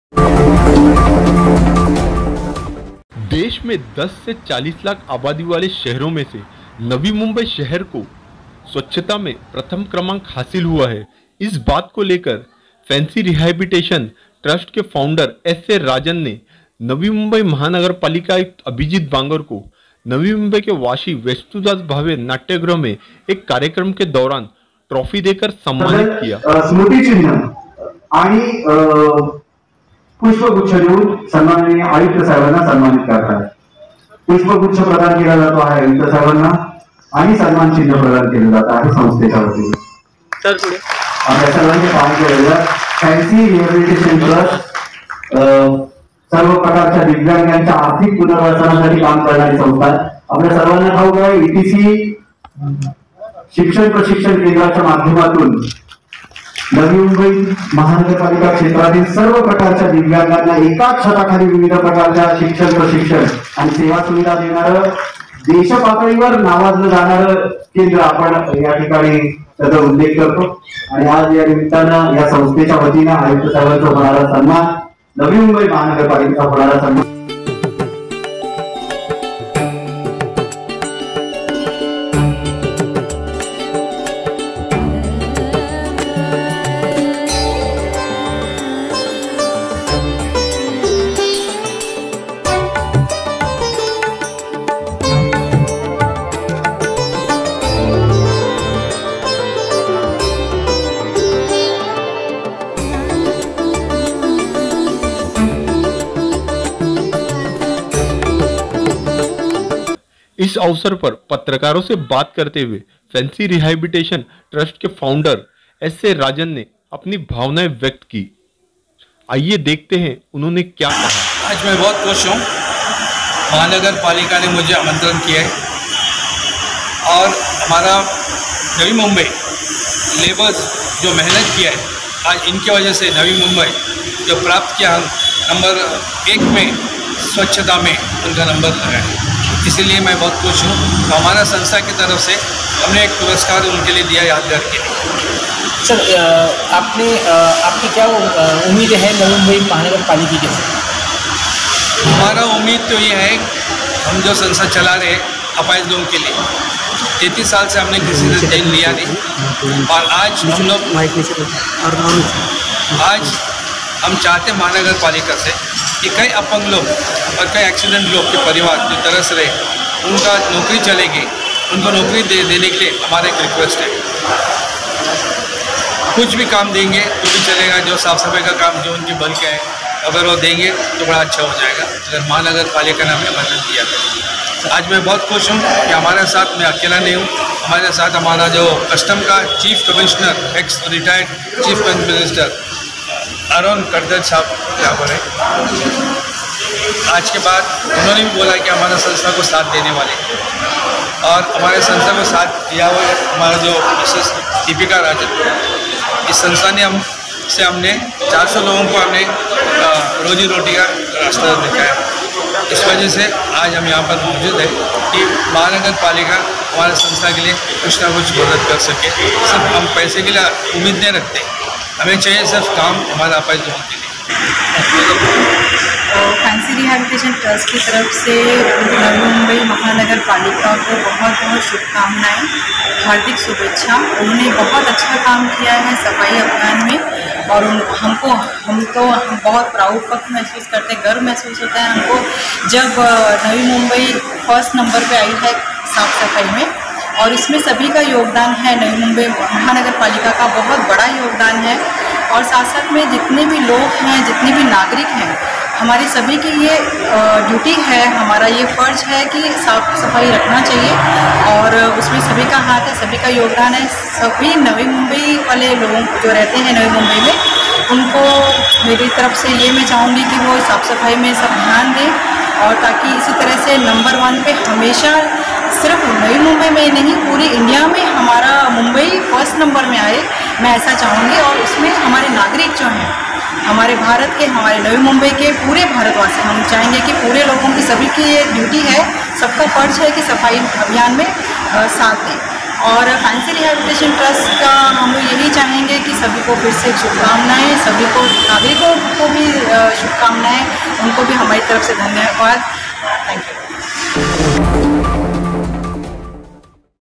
Fancy Rehabilitation Trust Founder S.A.Rajan मनपा आयुक्त बांगर को ट्रॉफी देकर सम्मान किया
SAYED.